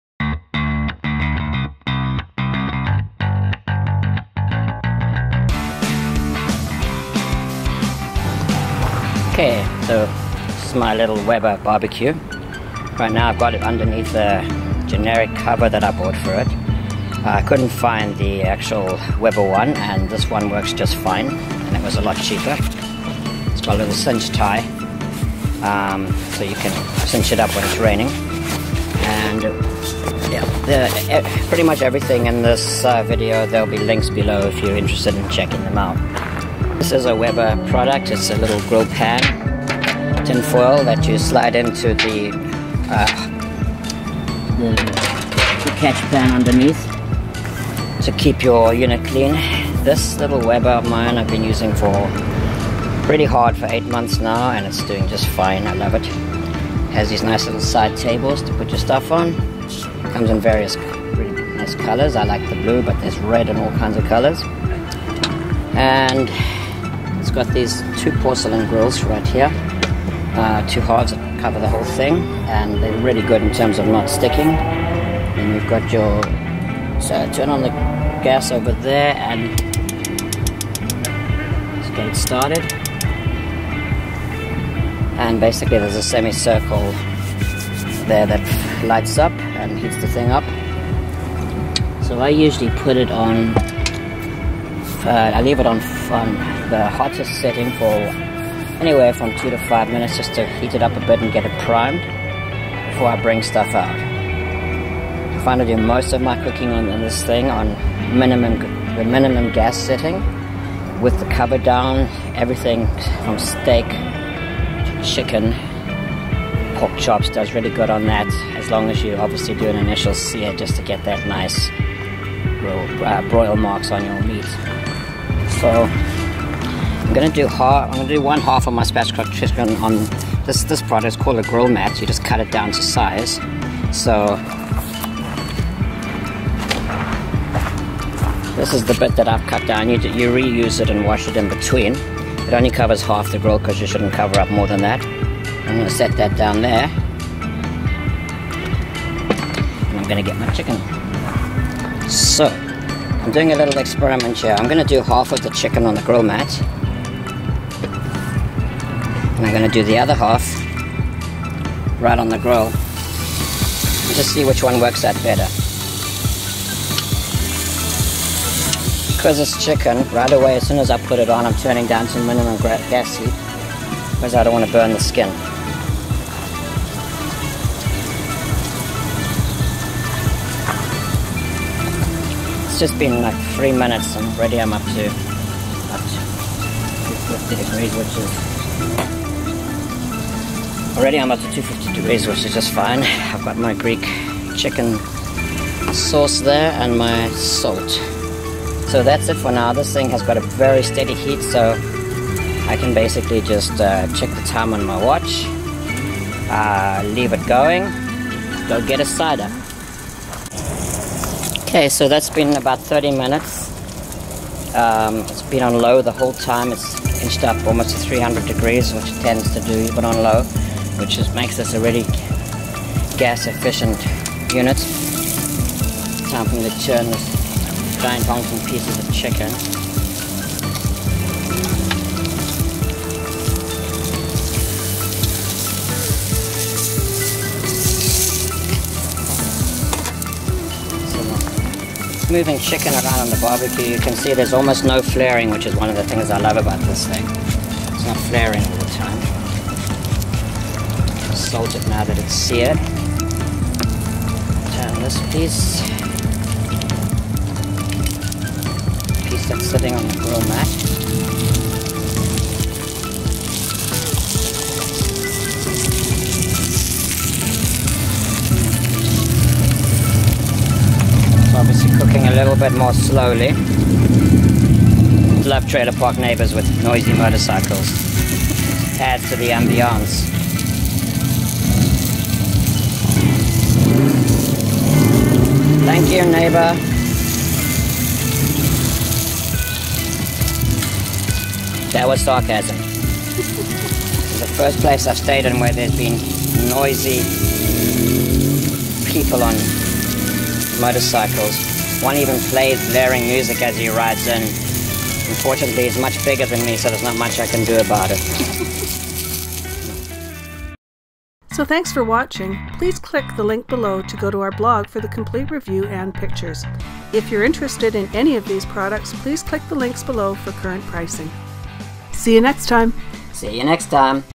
Weber Barbecue Review
Products in this video:
Weber Q1200 barbecue
Weber aluminum catch pans: h
Weber Q1000 series stand
Generic BBQ cover that fits all Weber Q1000 series small barbecues
Grill mats
Chickens: Find an egg and nurture it ...
Below is the music we used in the video. Our thanks to the musicians for composing and sharing!